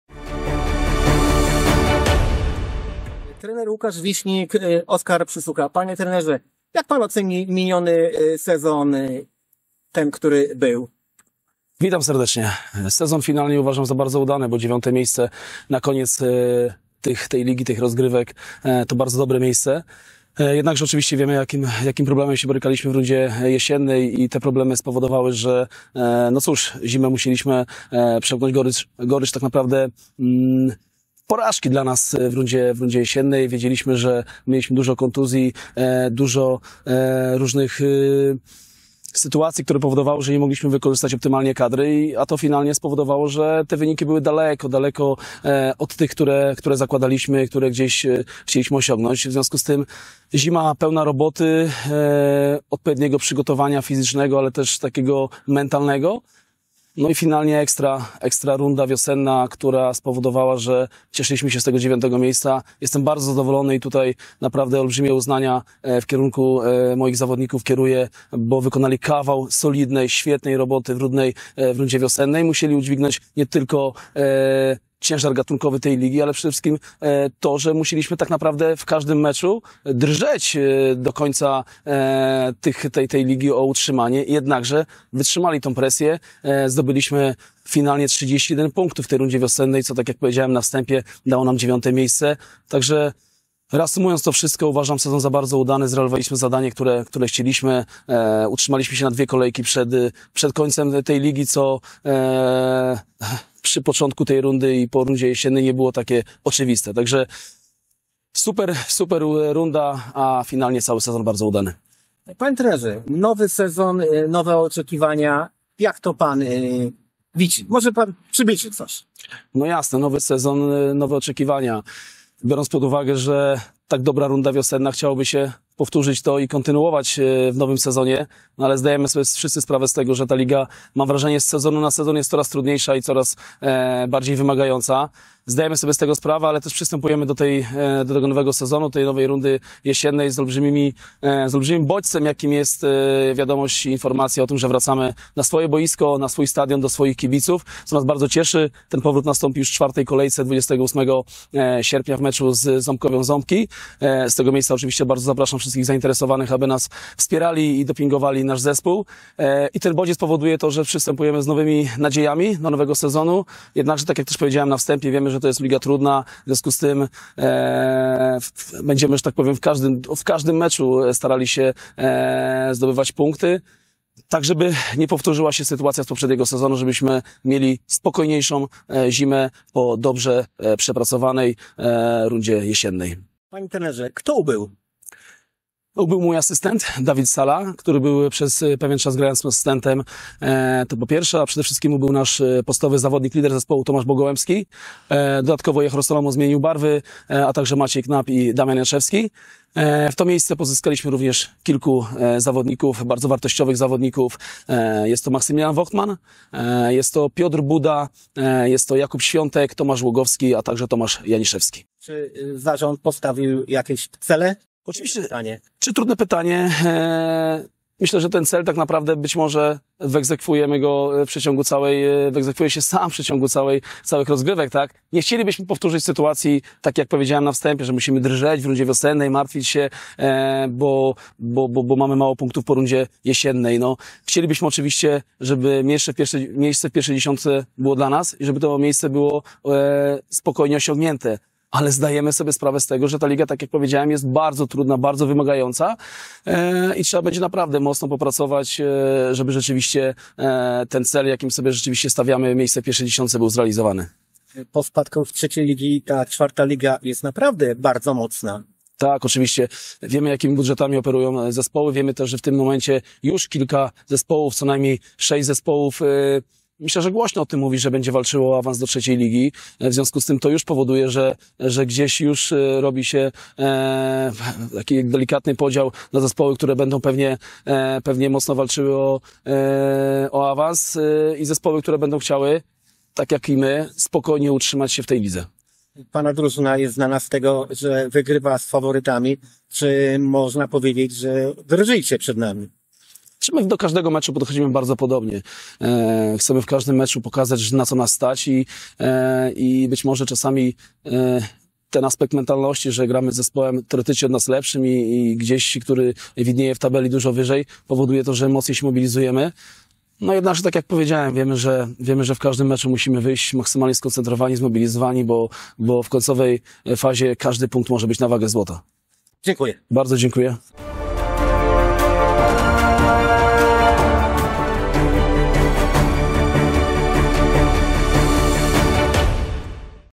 SKS Oskar Przysucha wywiad trener Lukasz Wiśnik
Ocena minionego sezonu .Jakie plany na 2025 rok